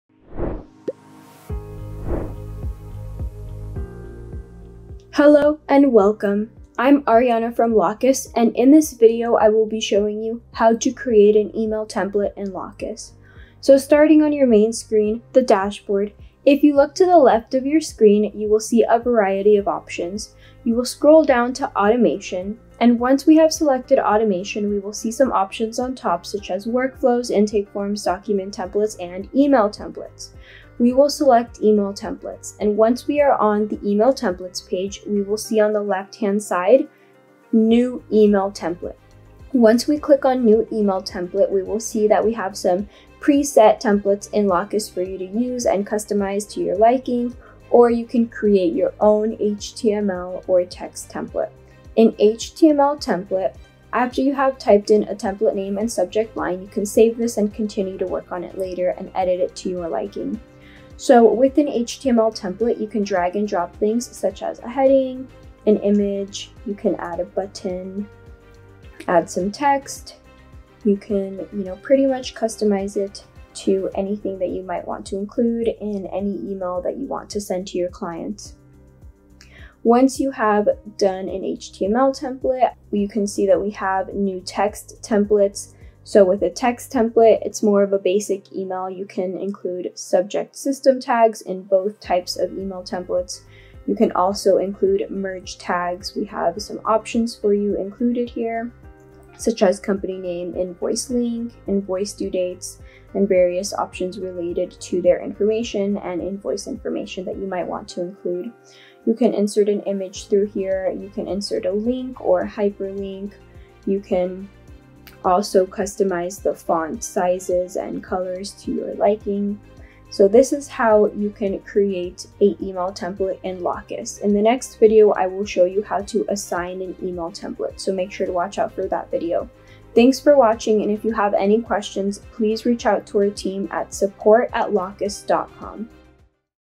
How to Create An Email Template is Lawcus
Description: Learn how to create the 2 types of email templates (HTML &  TXT) we have available in Lawcus with this short video tutorial.